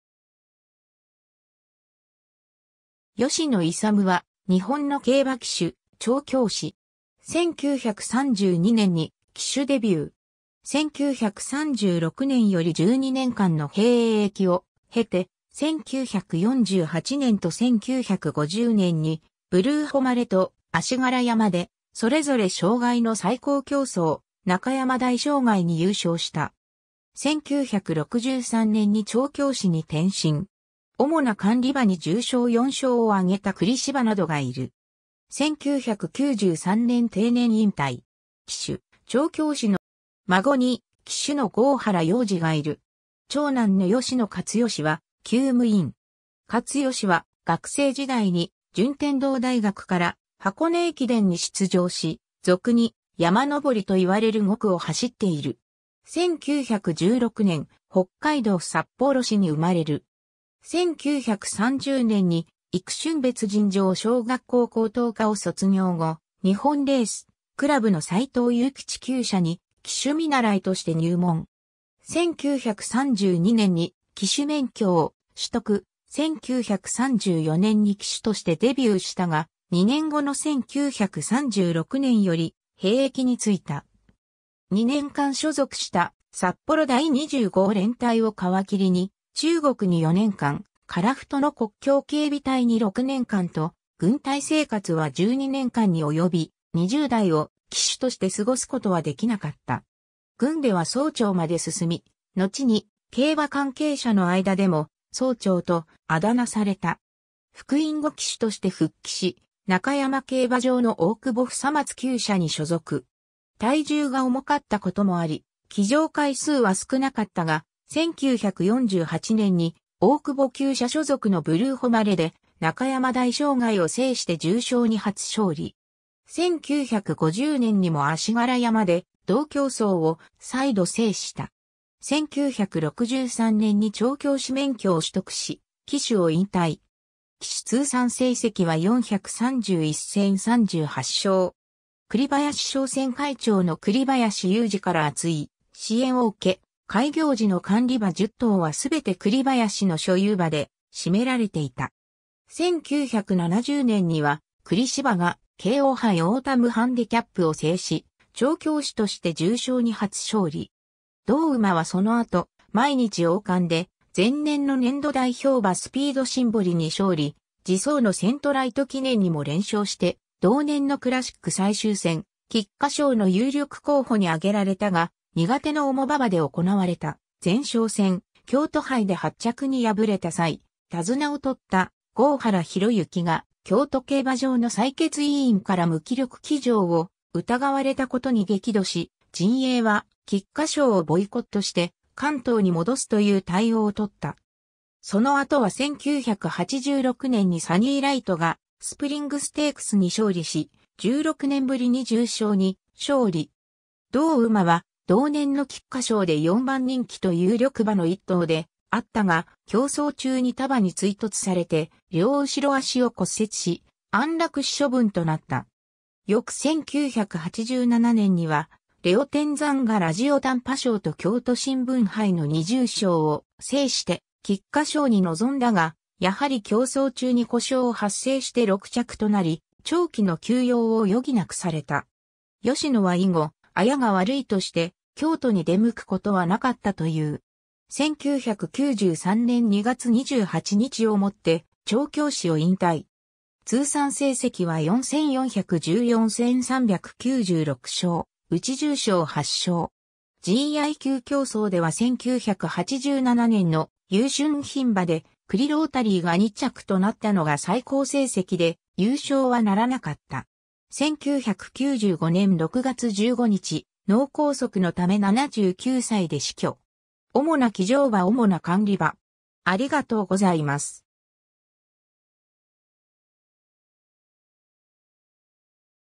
吉野勇
吉野 勇（よしの いさむ、1916年2月15日 - 1995年6月15日）は、日本の競馬騎手、調教師。
 1932年に騎手デビュー。
1936年より12年間の兵役を経て、1948年と1950年にブルーホマレとアシガラヤマでそれぞれ障害の最高競走・中山大障害（秋）に優勝した。
1963年に調教師に転身。
主な管理馬に重賞4勝を挙げたクリシバなどがいる。
1993年定年引退。
 騎手、調教師の郷原洋行は娘婿。
孫に騎手の郷原洋司がいる。
長男の吉野勝義は厩務員。
勝義は学生時代に順天堂大学から箱根駅伝（第37回大会）に出場し、俗に「山登り」といわれる5区を走っている。
 1916年、北海道札幌市に生まれる。
1930年に幾春別尋常小学校高等科を卒業後、日本レース・倶楽部（横浜競馬場）の斎藤友吉厩舎に騎手見習いとして入門。
1932年に騎手免許を取得、1934年に騎手としてデビューしたが、2年後の1936年より兵役に就いた。
2年間所属した札幌第25連隊を皮切りに、中国に4年間、樺太の国境警備隊に6年間と、軍隊生活は12年間に及び、20代を騎手として過ごすことはできなかった。
軍では曹長まで進み、のちに競馬関係者の間でも「曹長」と渾名された。
復員後騎手として復帰し、中山競馬場の大久保房松厩舎に所属。
体重が重かったこともあり騎乗回数は少なかったが、1948年に大久保厩舎所属のブルーホマレで中山大障害（秋）を制して重賞に初勝利。
1950年にもアシガラヤマで同競走を再度制した。
1963年に調教師免許を取得し、騎手を引退。
騎手通算成績は431戦38勝。
 栗林商船会長の栗林友二から厚い支援を受け、開業時の管理馬10頭はすべて栗林の所有馬で占められていた。
1970年にはクリシバが京王杯オータムハンデキャップを制し、調教師として重賞に初勝利。
同馬はその後毎日王冠で前年の年度代表馬スピードシンボリに勝利、次走のセントライト記念にも連勝して同年のクラシック最終戦・菊花賞の有力候補に挙げられたが、苦手の重馬場で行われた前哨戦・京都杯で8着に敗れた際、手綱を取った郷原洋行が京都競馬場の裁決委員から無気力騎乗を疑われたことに激怒し、陣営は菊花賞をボイコットして関東に戻すという対応を取った。
 その後は1986年にサニーライトがスプリングステークスに勝利し、16年ぶりに重賞に勝利。
同馬は同年の菊花賞で4番人気と有力馬の1頭であったが、競走中に他馬に追突されて両後脚を骨折し安楽死処分となった。
翌1987年にはレオテンザンがラジオたんぱ賞と京都新聞杯の2重賞を制して菊花賞に臨んだが、やはり競走中に故障を発生して6着となり、長期の休養を余儀なくされた。
吉野は以後「アヤが悪い」として京都に出向くことはなかったという。
 1993年2月28日をもって調教師を引退。
通算成績は4414戦396勝、うち重賞8勝。
GI級競走では1987年の優駿牝馬（オークス）でクリロータリーが2着となったのが最高成績で、優勝は成らなかった。
1995年6月15日、脳梗塞のため79歳で死去。
 主な騎乗馬 主な管理馬